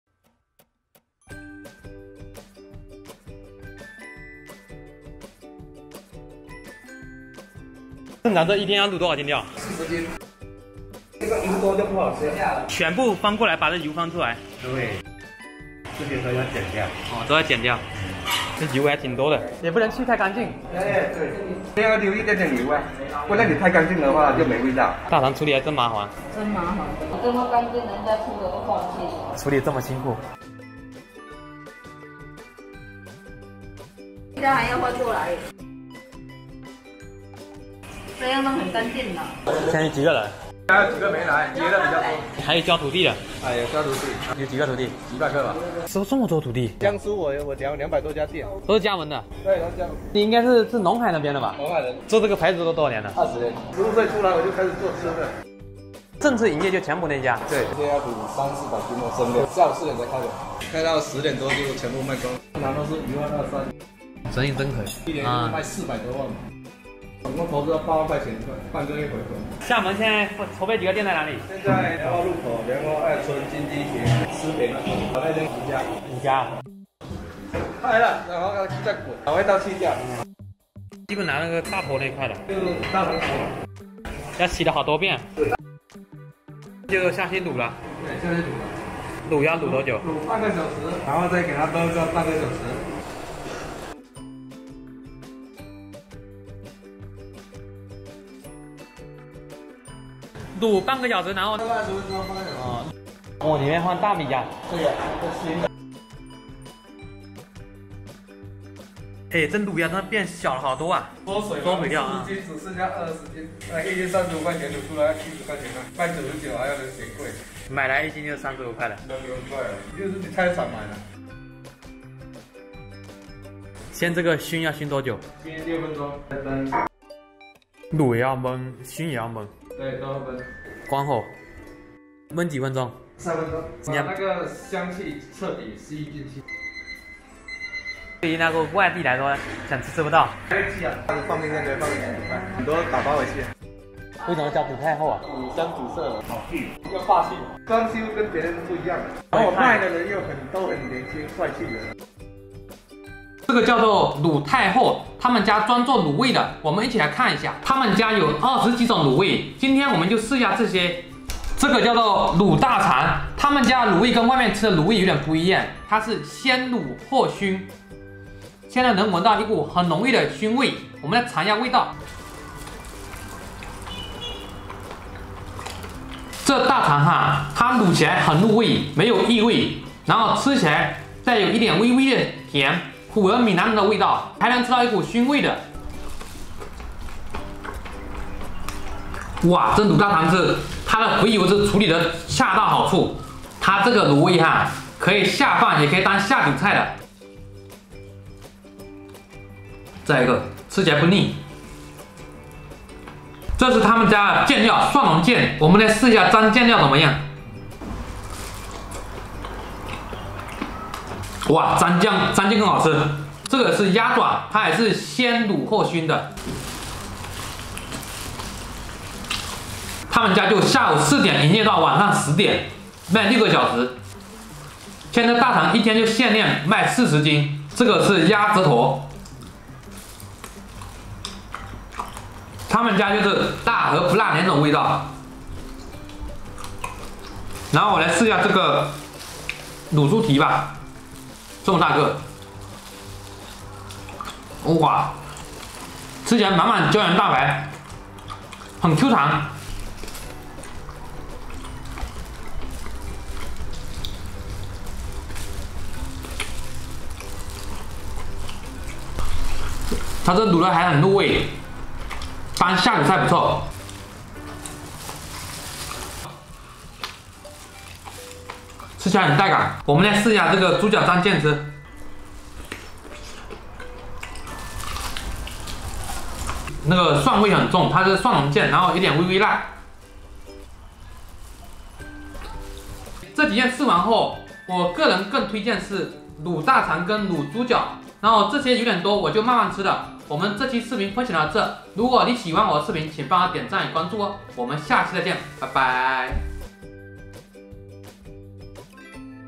福建大哥做小吃20年，每天卖400斤卤肉只营业6小时，年入400多万
在中国有名的卤味店非常多，而且各个地方的口味都不相同，今天我们来说下闽南的卤味，闽南人非常爱吃卤味，无论是早餐的加料、午餐的正菜还是晚餐的下酒菜，而且还很喜欢吃猪内脏的卤味——大肠、小肠、猪肺等等。闽南地区的卤味整体上是咸中带甜，香气十足。
  今天要分享的是一位漳州龙海大哥创业做卤味，他靠12平米的小店投资8万元，开业半个月就回本了。每天只营业6个小时，一年能卖至少400万。他们家做的卤味是闽南口味的，但是跟以往吃的卤味又有点不同，他们是先卤后熏。吃起来有卤味的香味，还有一股熏烤的香味。他们家的卤大肠的肥油处理得恰到好处，每天限量做40斤，先处理干净，剪掉大部分的肥油，下锅焯水一下，再清洗一遍，才下锅卤。卤完还需再熏烤一遍，放凉才算大功告成。个人觉得这味道不错，而且靠着一手卤味的手艺征服了上百个徒弟。